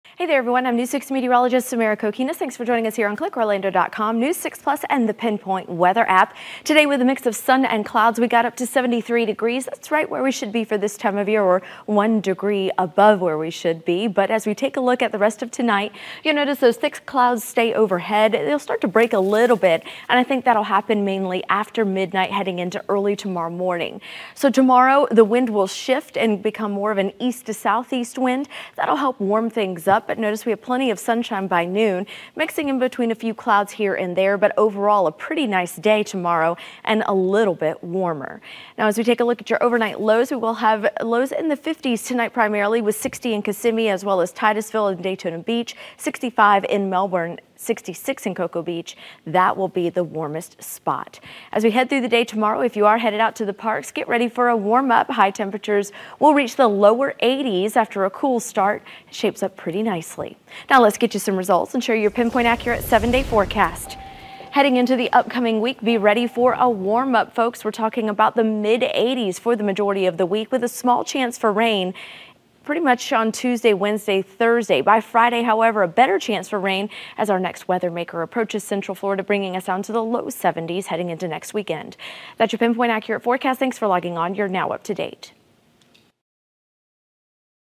Evening weather forecast for January 28
WKMG News 6 Meteorologist Samara Cokinos provides the pinpoint Central Florida weather forecast for Saturday evening, January 28, 2023